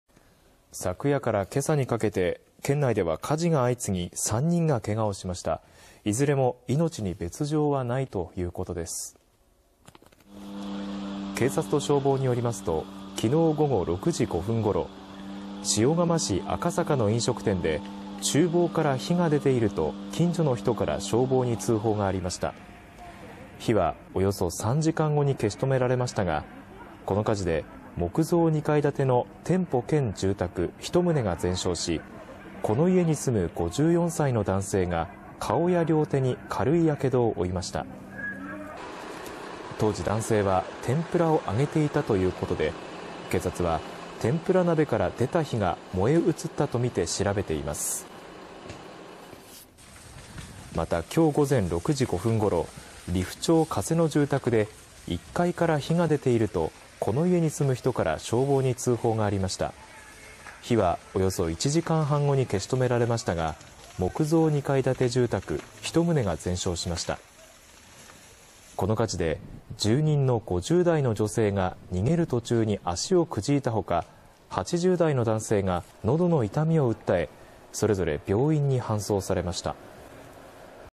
宮城県の塩釜市と利府町で火事が相次ぐ 計３人けが
７日夜から８日朝にかけて宮城県では火事が相次ぎ、３人がけがをしました。いずれも命に別状はないということです。